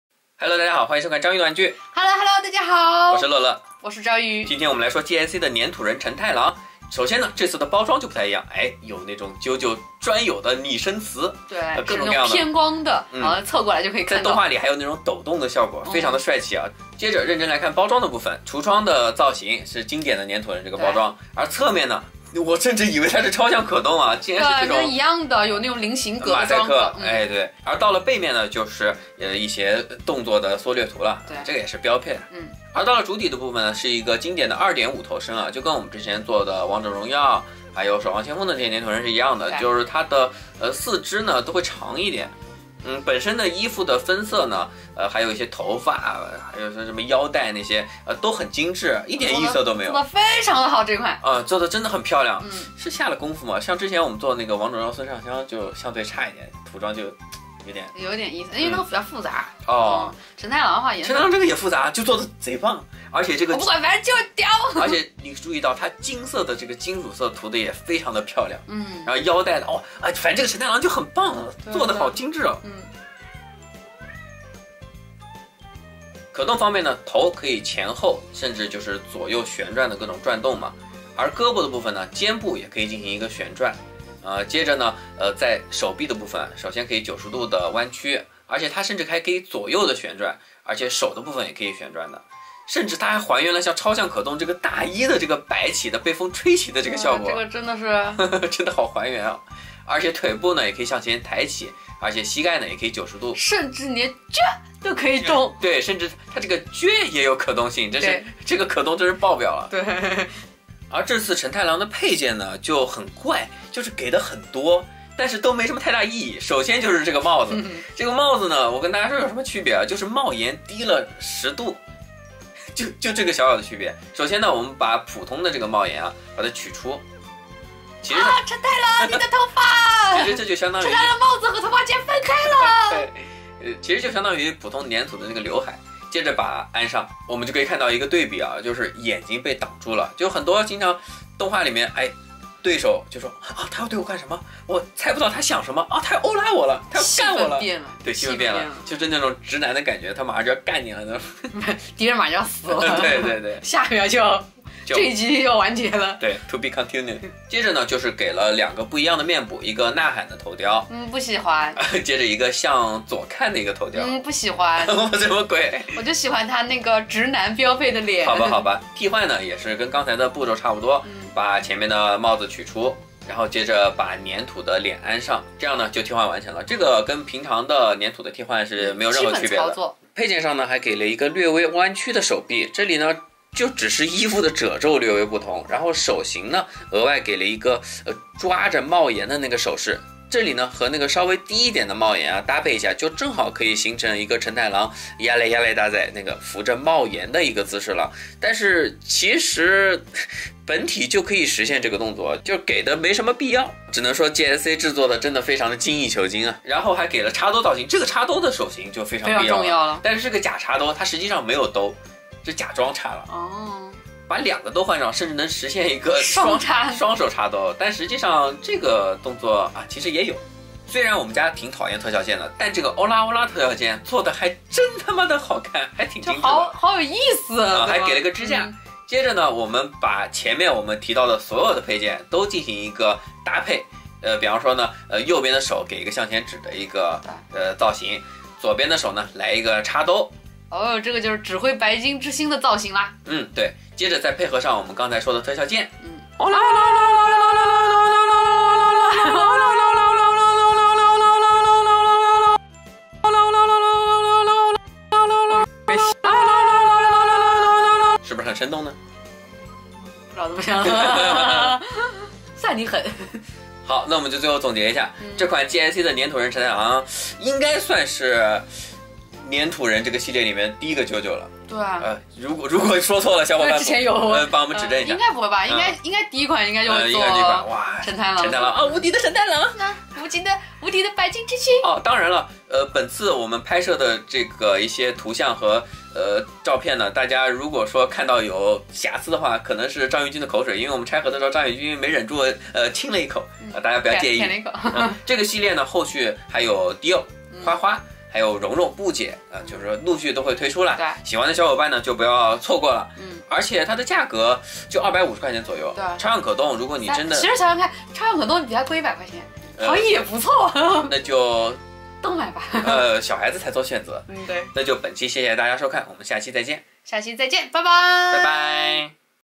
可爱的卖鱼强！GSC 粘土人 JOJO的奇妙冒险 空条承太郎 开盒简评！【章鱼的玩具】
又可爱，又帅气~喔啦喔啦喔啦喔啦~